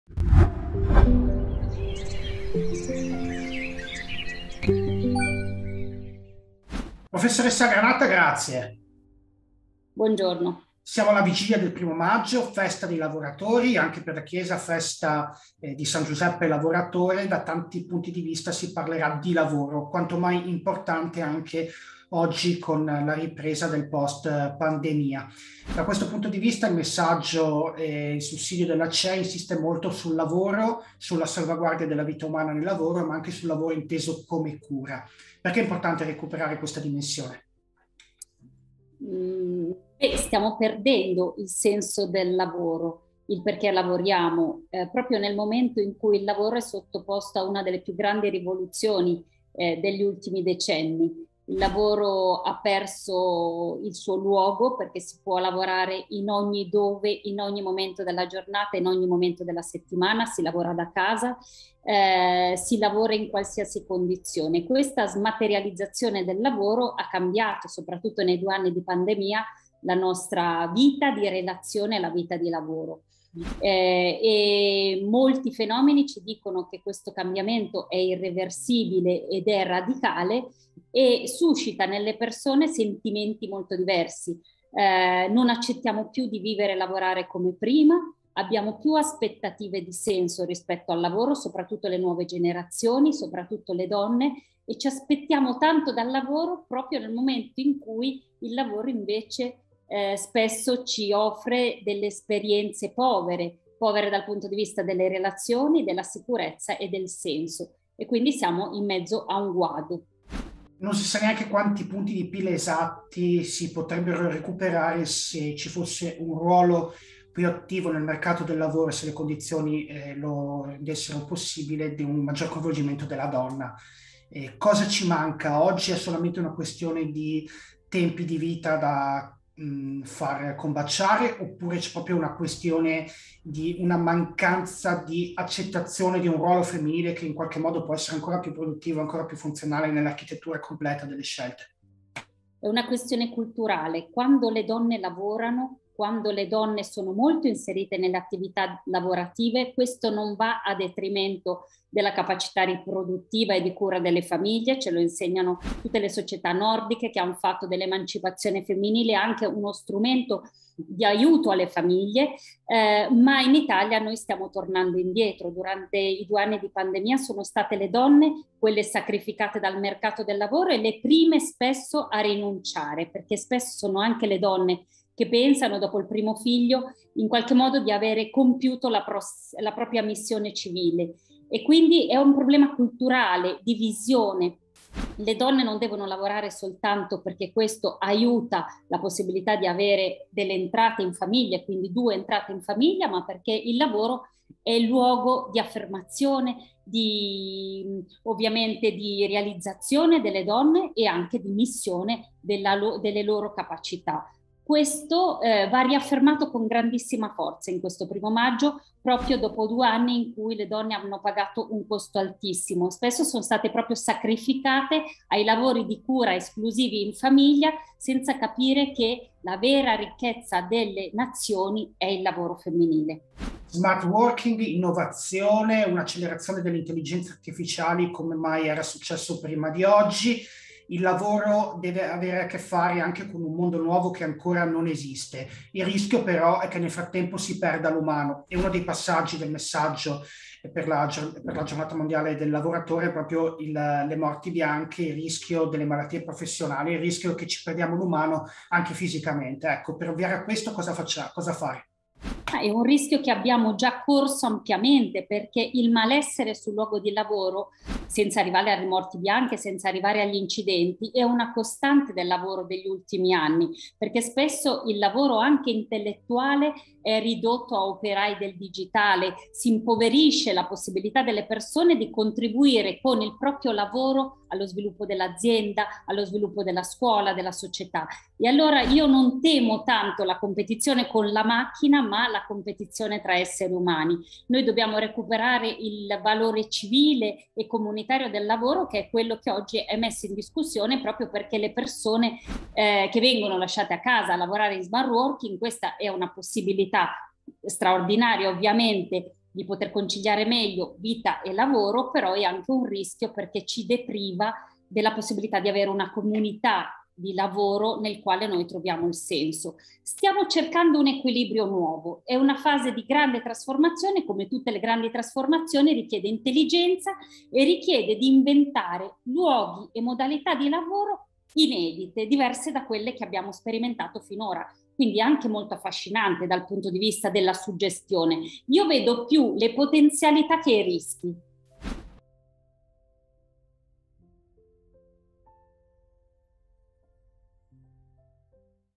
Perché lavoriamo? Il senso del lavoro e il senso del lavoro femminile. Con Elena Granata
🌿 Alla vigilia del primo maggio, Festa di San Giuseppe Lavoratore, ci interroghiamo sul significato più profondo del lavoro, per dare "senso" a ciò che facciamo. Con con la prof.ssa Elena Granata del Comitato promotore delle Settimane Sociali dei Cattolici Italiani scopriamo cosa può rappresentare il lavoro per le donne, anche di fronte alle sfide della modernità.

📣 Leggi il sussidio liturgico e pastorale "La vera ricchezza sono le persone" della Cei